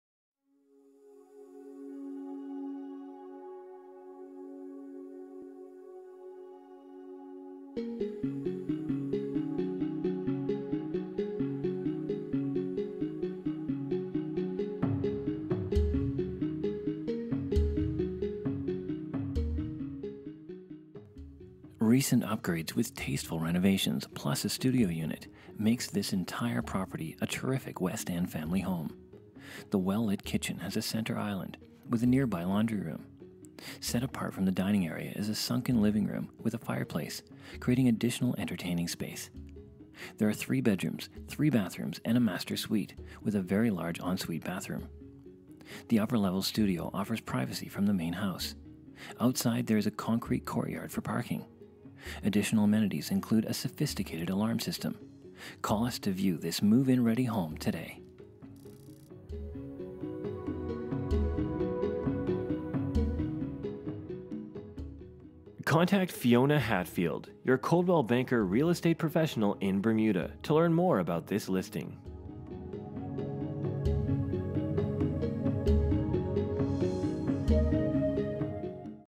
God Sent
Recent upgrades with tasteful renovations, plus a studio unit, makes this entire property a terrific West End family home. The well-lit kitchen has a center island with a nearby laundry room. Set apart from the dining area is a sunken living room with a fireplace, creating additional entertaining space. There are three bedrooms, three bathrooms and a master suite with a very large ensuite bathroom. The upper level studio offers privacy from the main house. Outside, there is a concrete courtyard for parking. Additional amenities include a sophisticated alarm system. Call us to view this move-in ready home.